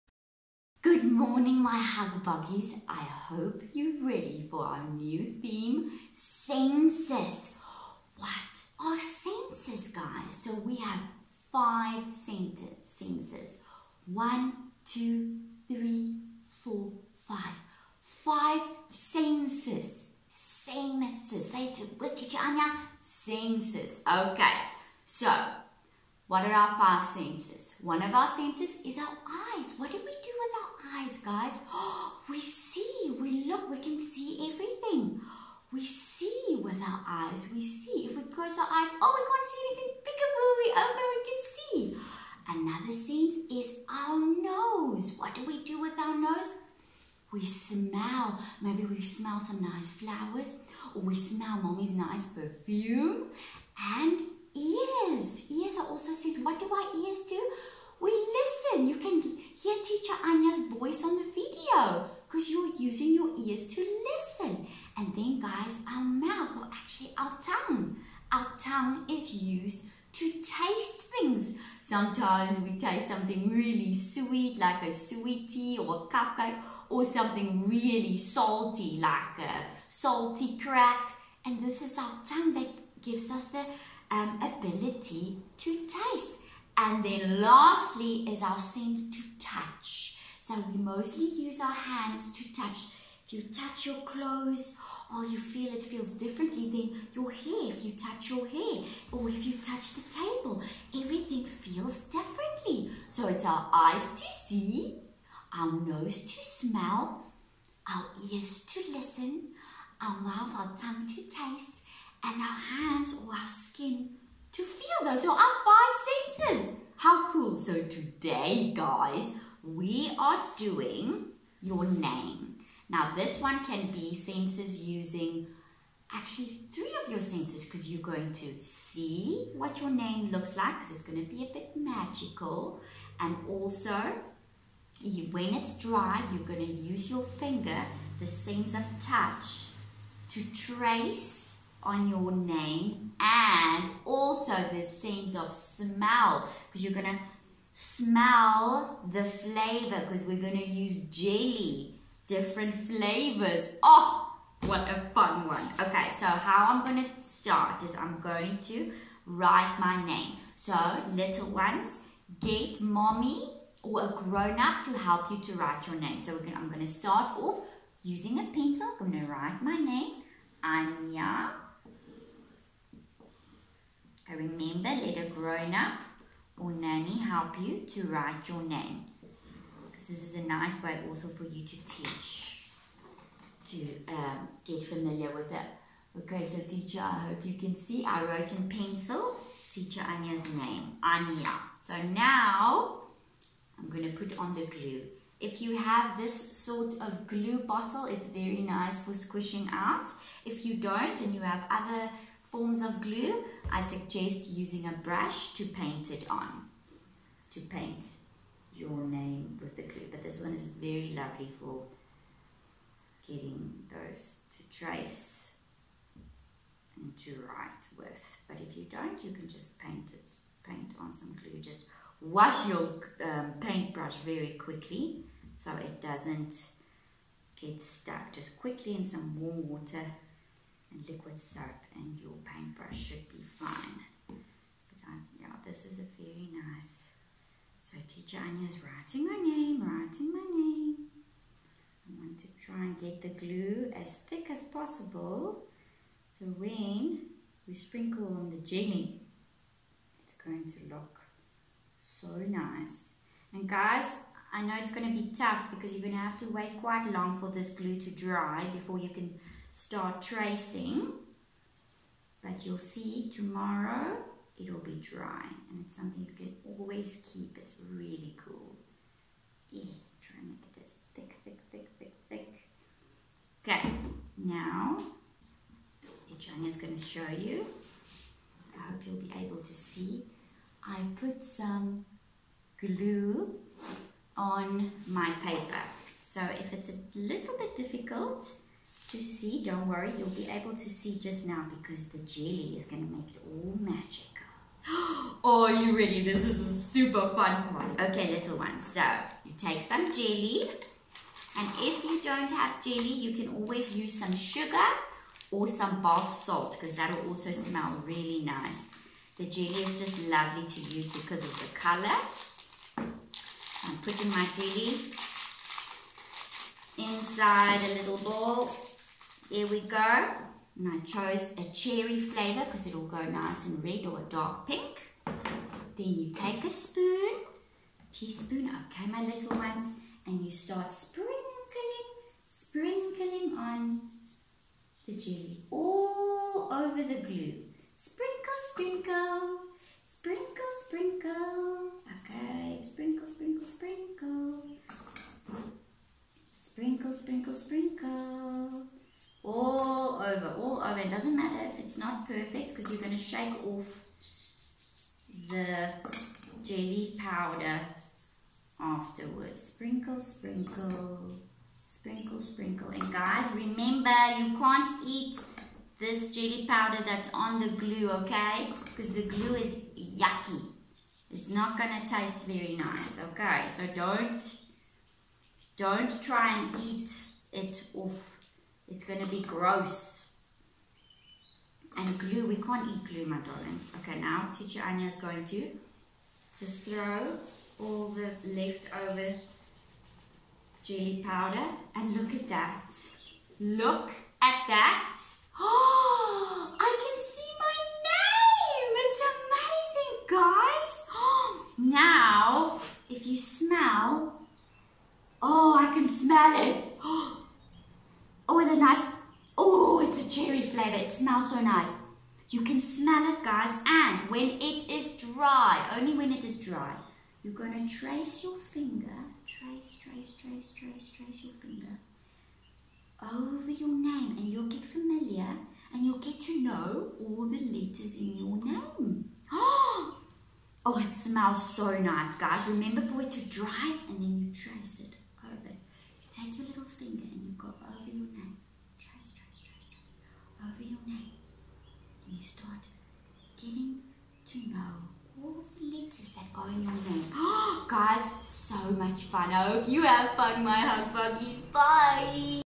Toddler Acyivities / Crafts: Glue Jelly Name
All you need:
-white cardboard paper
-pencil
-Craft glue
-Flavoured Jelly powder

Have fun smelling and tracing your names little ones!

Love
Teacher Anja